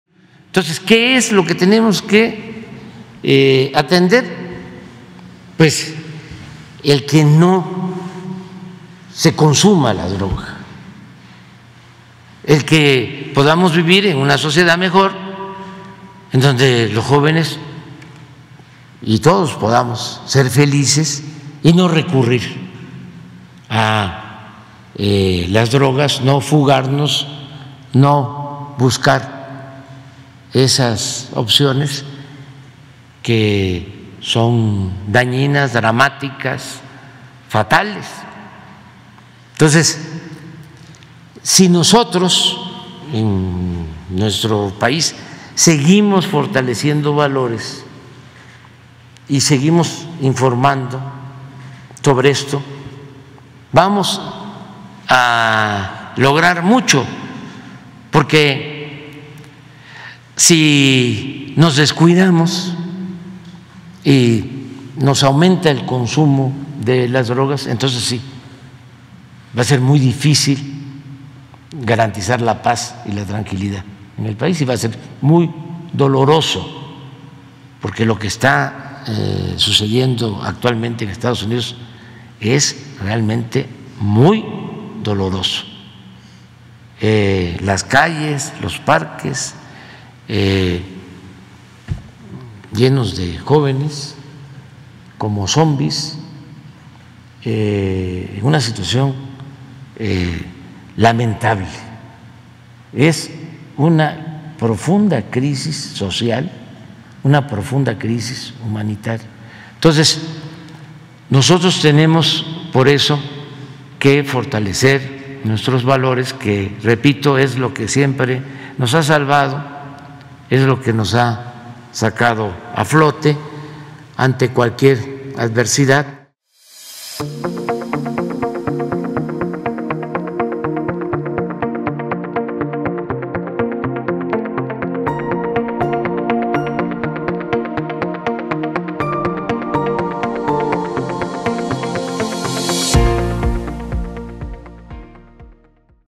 AMLO dice que valores culturales protegen del fomento al consumo de drogas en México
El mandatario reiteró la necesidad que hay Estados Unidos de tener una atención social al problema de consumo de drogas que actualmente enfrenta; AMLO planteó colaboración con el Consejo de la Comunicación para la campaña de prevención en México.